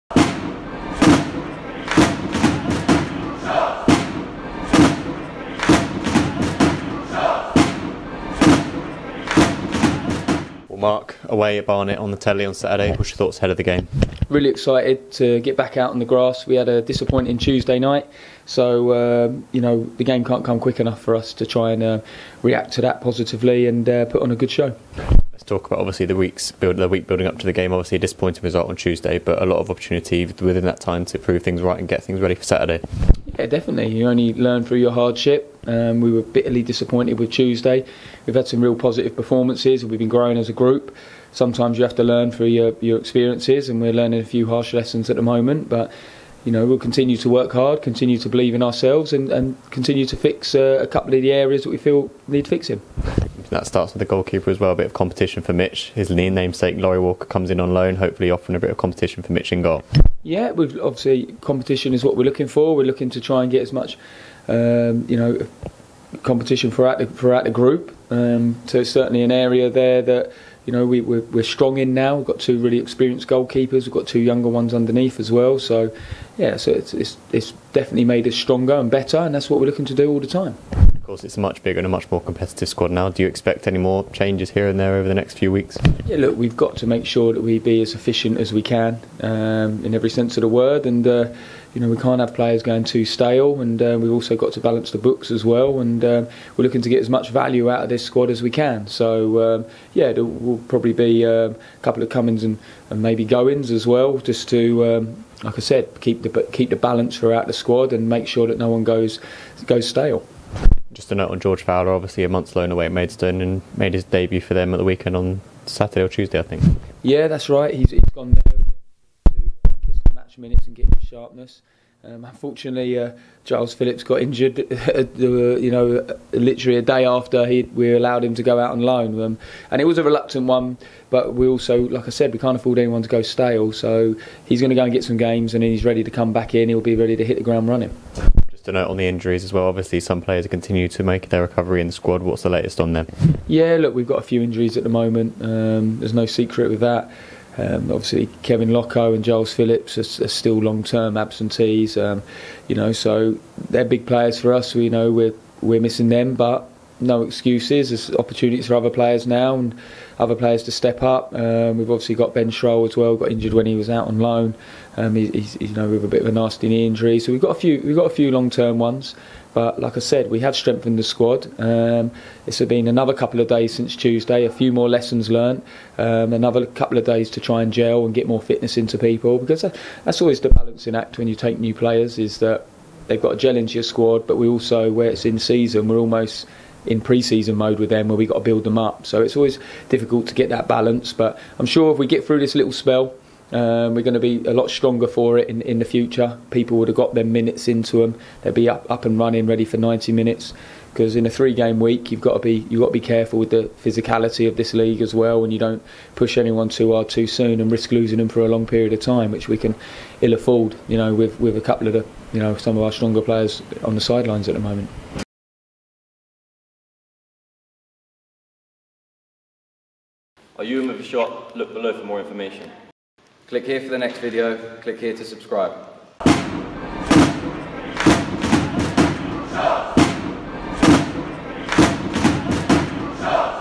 Mark Molesley Pre-Match: Barnet FC (A)
Mark Molesley looks ahead to tomorrow's Vanarama National League away match against Barnet FC live on BT Sport